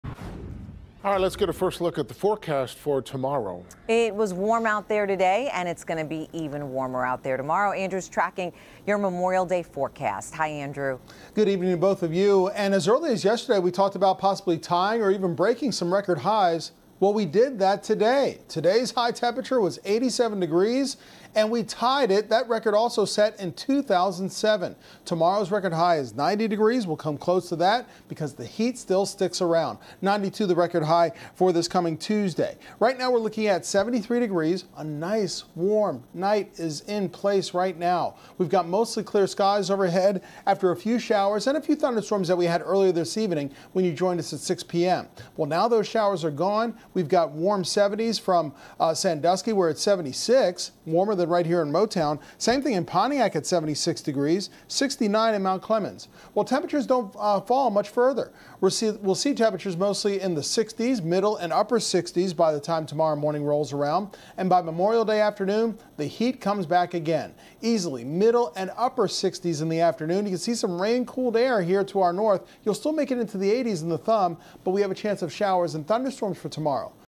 Metro Detroit weather: Humid Sunday night, May 24, 2020, 11 p.m. update
Scattered showers and storms from the late afternoon and evening are departing and dissipating. A southerly wind continues the flow of summer-like air overnight and Memorial Day. Temperatures skyrocket, Monday afternoon, as scattered showers and storms re-form.